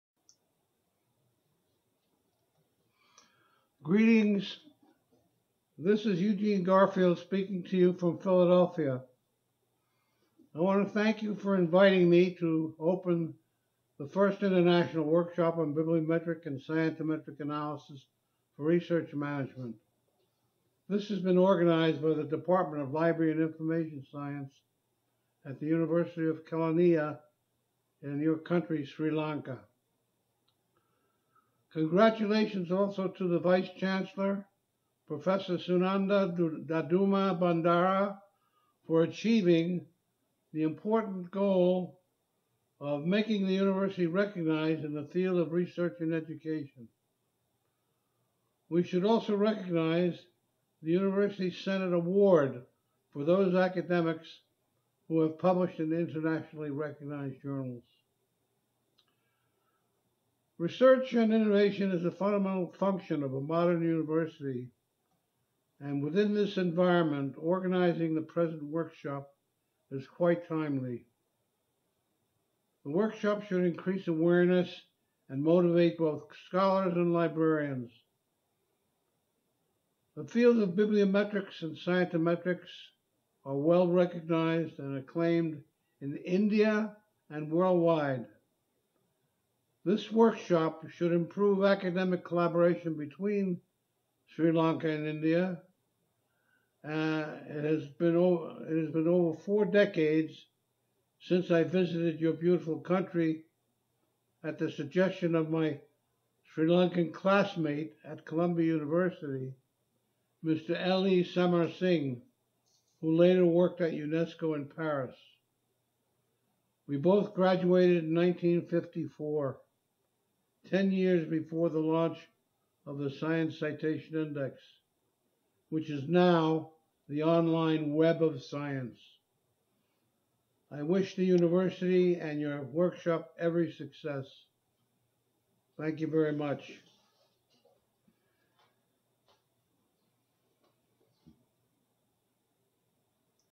Eugene Garfield, Sri Lanka Message, July 2015
Message from Eugene Garfield, Founder & President, Institute for Scientific Information, on the occasion of the First International Workshop on Bibliometric & Scientometric Analysis for Research Management, Sri Lanka, July 2015